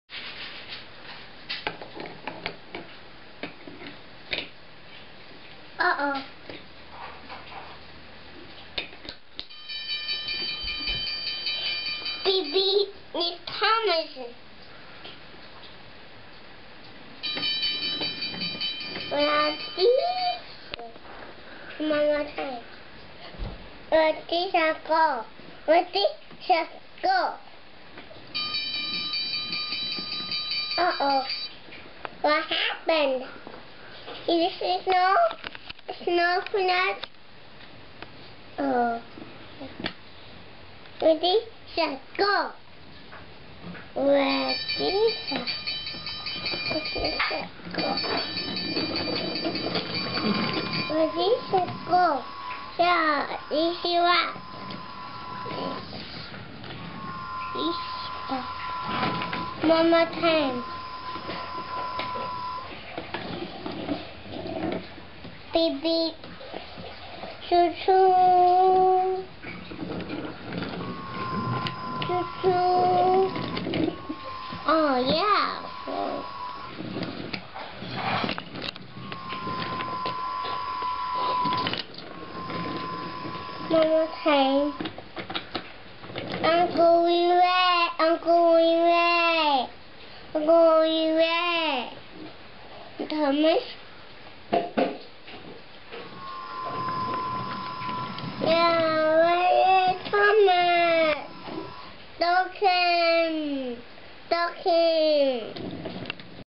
25m toddler play with train table
Logan (2 year 1 month) plays with train table by himself while talking to himself. He says "one more time", "what happened", "uh oh", "I'm going away", "snow", "Thomas", "Ducan", "oh yeah".